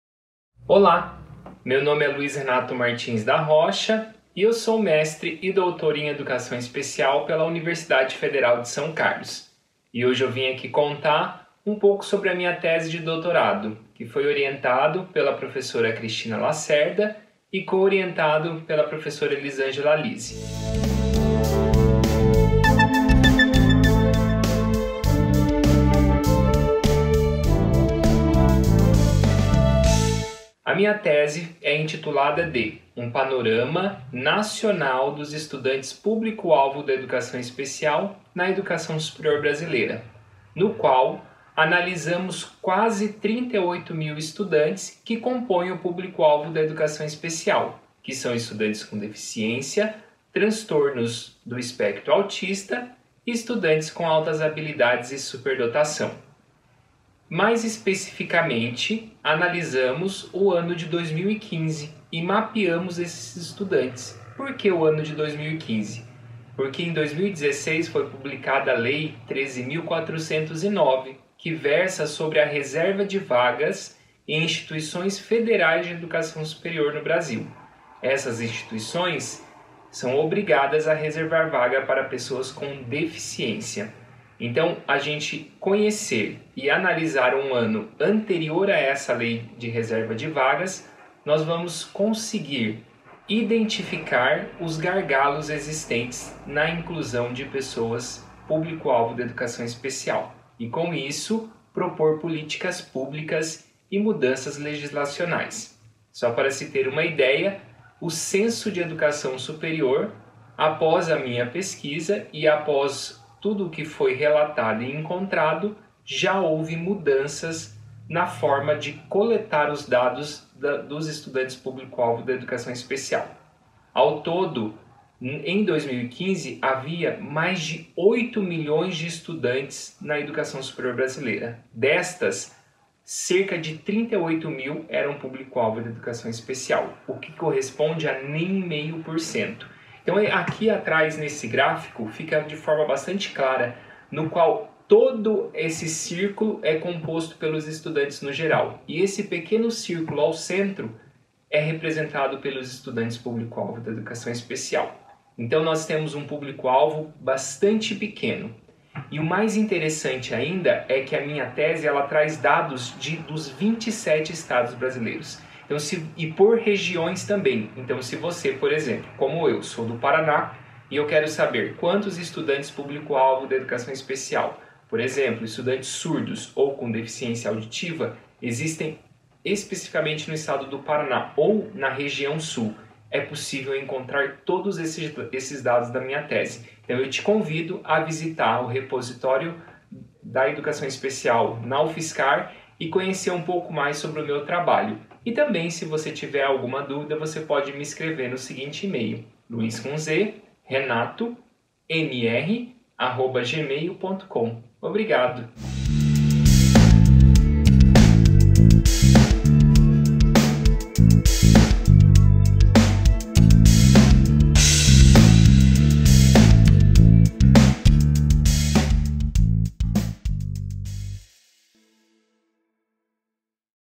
Mapeamento de estudantes público-alvo da educação especial é tema de pesquisa na UFSCar
Luiz Renato Martins da Rocha, doutor pelo Programa de Pós-Graduação em Educação Especial pela Universidade Federal de São Carlos (PPGEEs - UFSCar), fala de sua pesquisa sobre o panorama nacional dos estudantes público-alvo da educação especial na educação superior brasileira por meio do mapeamento durante o ano de 2015.